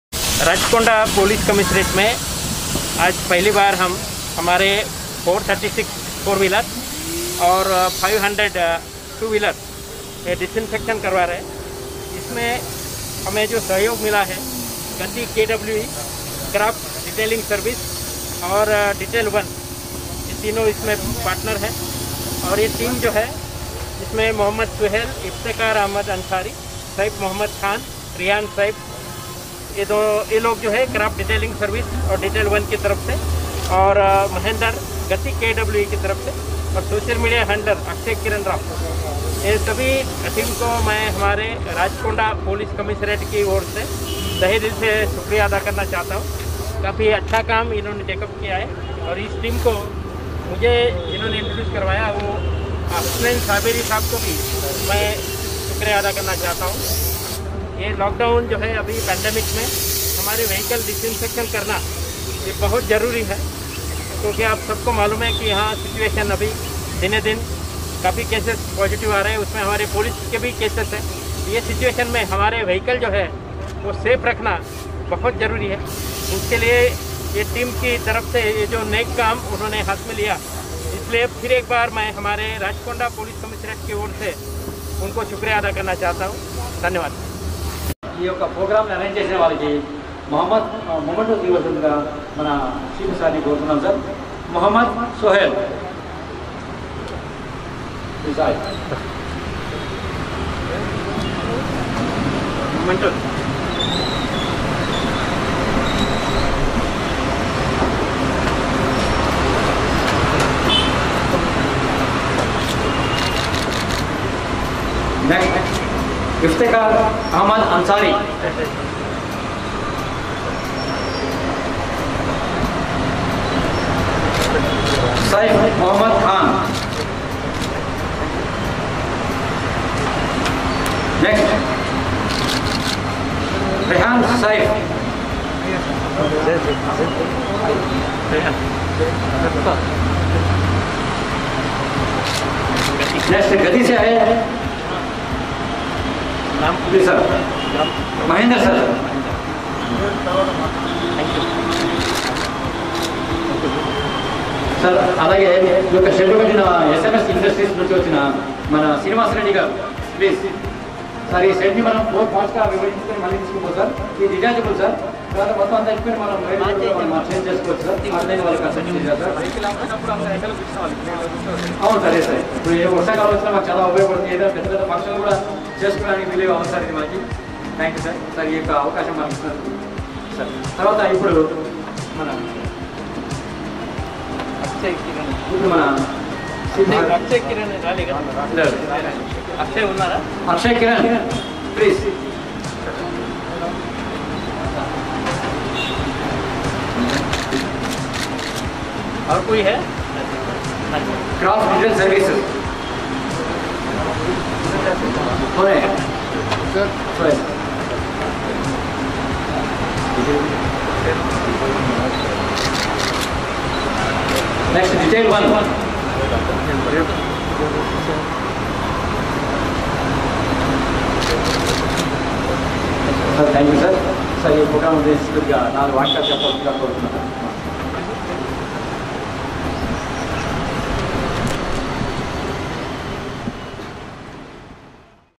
Craft_Detailing_Services in association with Gati Limited has started a Disinfection &
Craft_Detailing_Services in association with Gati Limited has started a Disinfection & Sanitization drive for the vehicular fleet of Rachakonda Police Commissionerate till 3 days under the guidance of CP_Rachakonda, Sri Mahesh M Bhagwat, IPS at AmberpetPoliceGrounds...